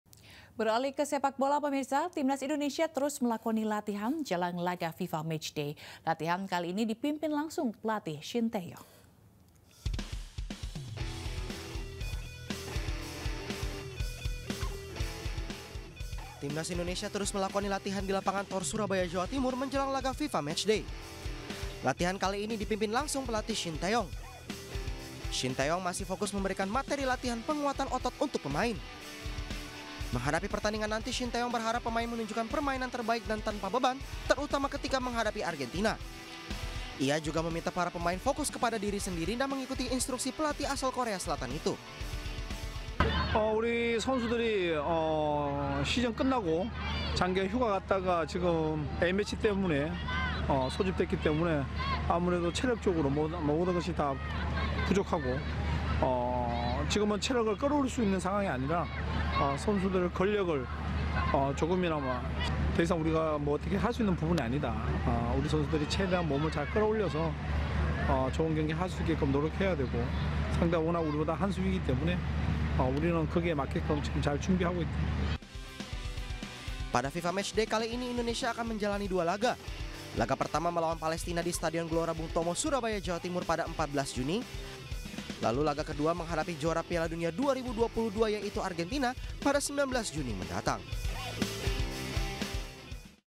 Timnas Indonesia Bersiap Jelang Fifa Matchday, STY Fokus pada Penguatan Fisik Skuad
Timnas Indonesia saat ini terus melakoni latihan di Lapangan Thor, Surabaya jelang menghadapi dua laga FIFA Matchday melawan Palestina (14/6) dan Argentina (19/6) mendatang. Pelatih Shin Tae-yong yang baru memimpin latihan hari ini masih fokus memberikan materi latihan penguatan otot untuk pemain.